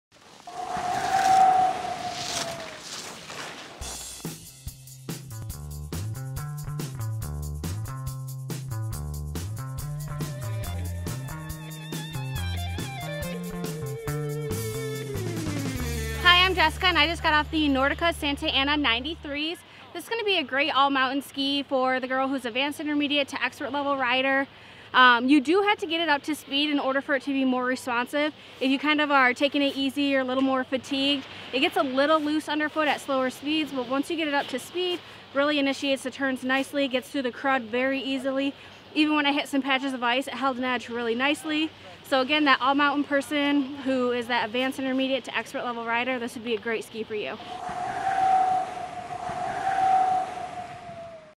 Jessica's Review-Nordica Santa Anna 93 Skis 2019-Skis.com
The Nordica Santa Ana 93 is one ripping ride for ladies looking to take on the entire mountain at full throttle. Nordica's Energy 2 Titanium Balsa Construction is packed with power that keeps you ultra-stable at high speeds. Balsa Wood is used in the core as a perfect weight reduction technique. Balsa Wood is known for its incredibly high strength to weight ratio and its sustainability. A Blunt Nose Profile improves floatation in deep snow, absorbs negative vibrations caused by bumps and crud, and delivers a smooth entry into your turns on the frontside of the mountain. An All-Mountain camRock Rocker Profile makes your skiing easy in any condition or terrain. If you are an aggressive skier looking to take on the entire mountain, the Nordica Santa Ana 93 is a perfect choice.